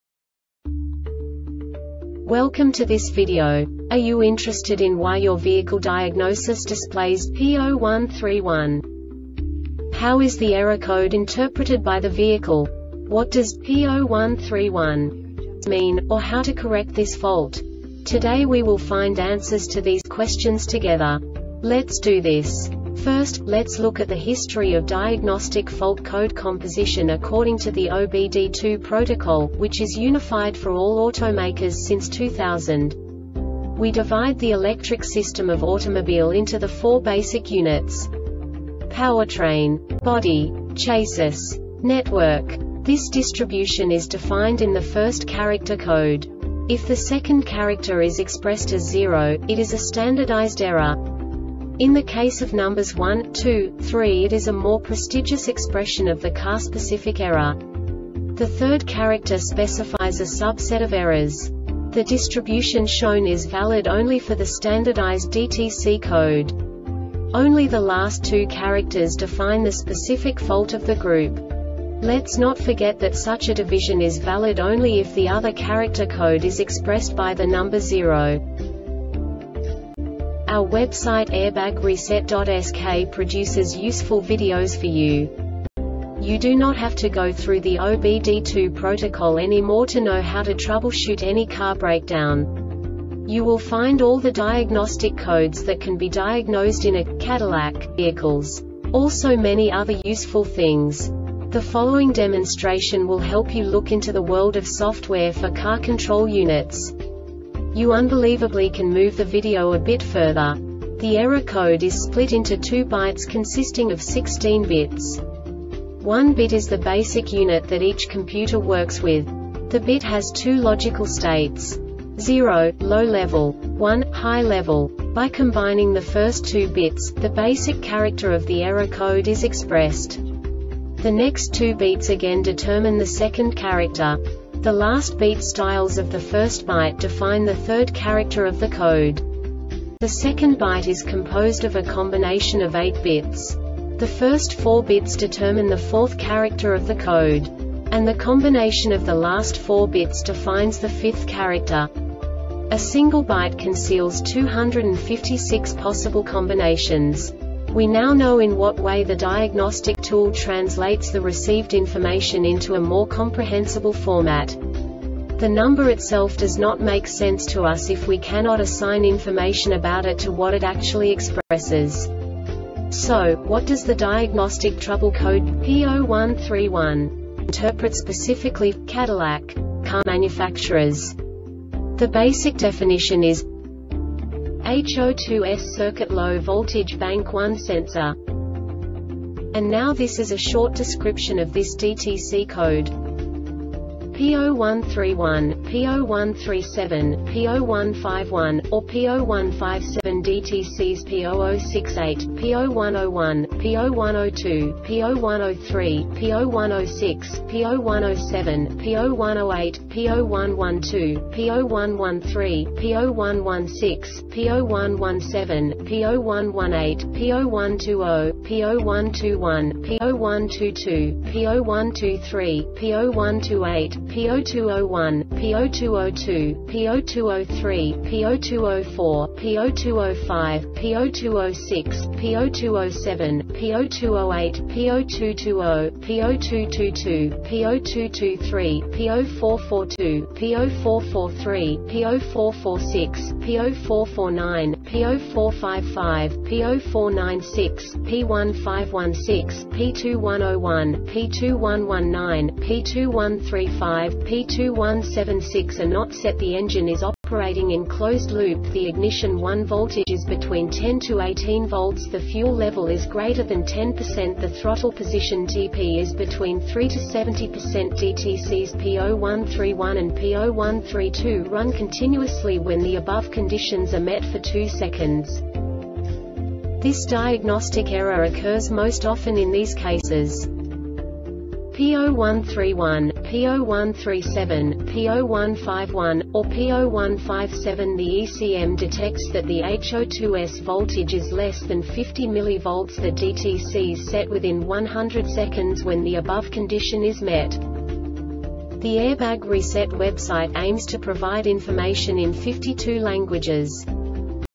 DTC cadillac P0131 Short Explanation
Contents:
0:21 Basic DTC analysis according to OBD2 protocol standard.
1:48 Insight into programming
2:58 A diagnostically understandable description of the Diagnostic Trouble Code
3:05 Basic error definition

Make:
Cadillac
Code:
P0131
Definition:
HO2S Circuit Low Voltage Bank 1 Sensor 1
Description:
Cause:
P0131, P0137, P0151, or P0157
The ECM detects that the HO2S voltage is less than 50 mV.
The DTCs set within 100 seconds when the above condition is met.